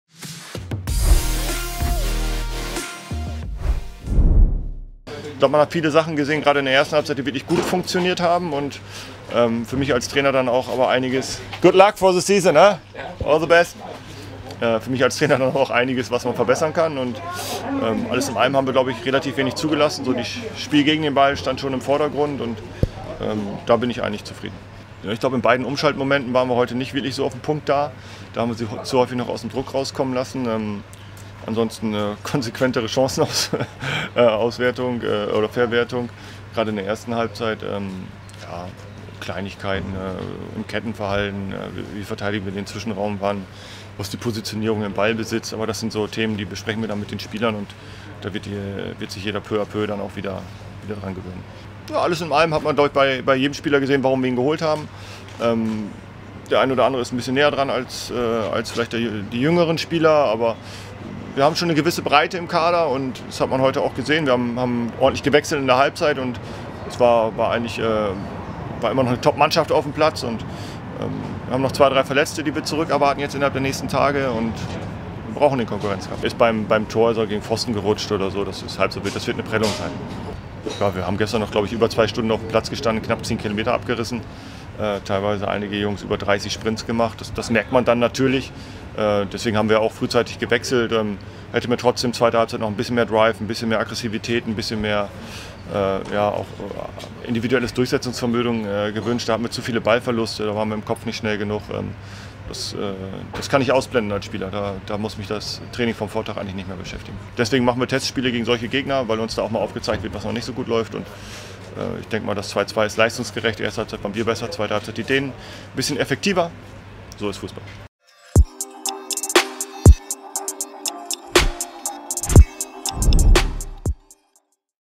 Die Spielanalyse | FC St. Pauli - Odense BK Testspiel
Leistungsgerechtes Unentschieden!

Timo Schultz, Trainer des FC St. Pauli ist zufrieden mit dem Testspiel gegen den dänischen Erstligisten Odense BK. Kurz vor dem Saisonbeginn bemängelt er nur die Chancenverwertung seiner Mannschaft.

Testspiel (10.07.21)